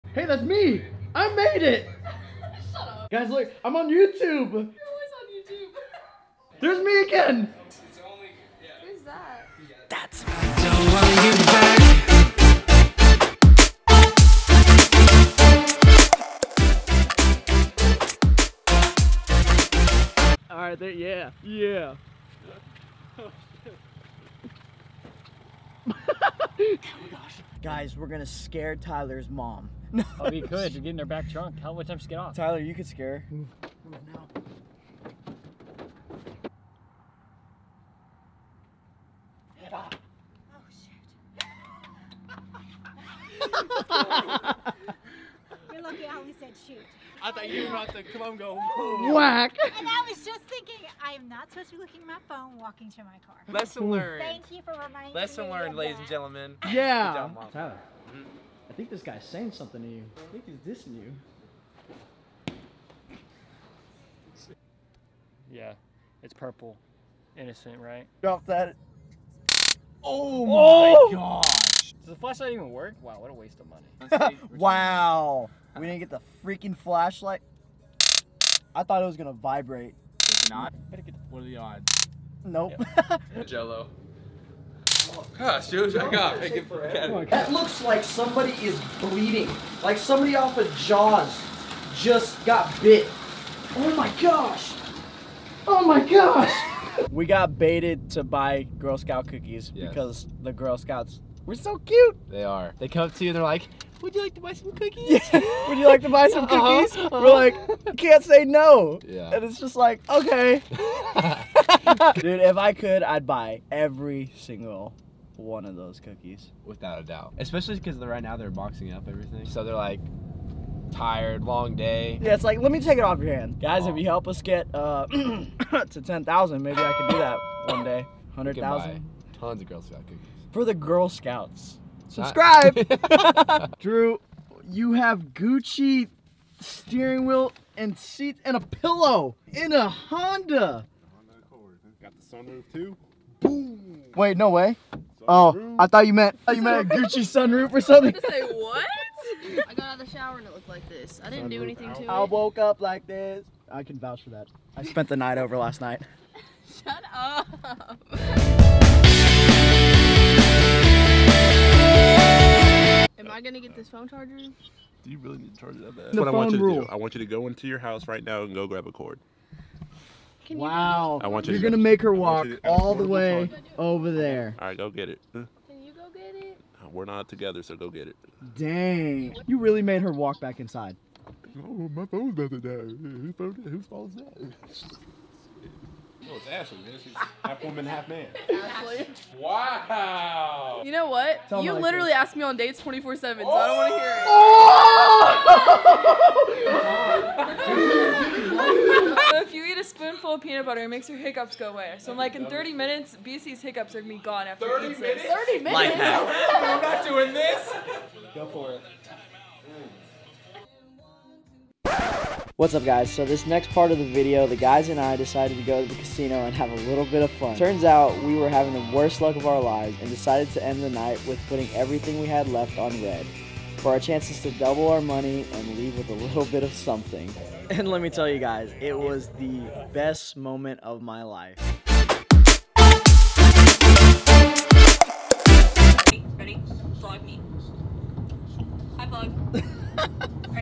PUTTING IT ALL ON RED!! ($$$$)
We decided to go have fun at the casino! Also seeing yourself on Youtube is very cool.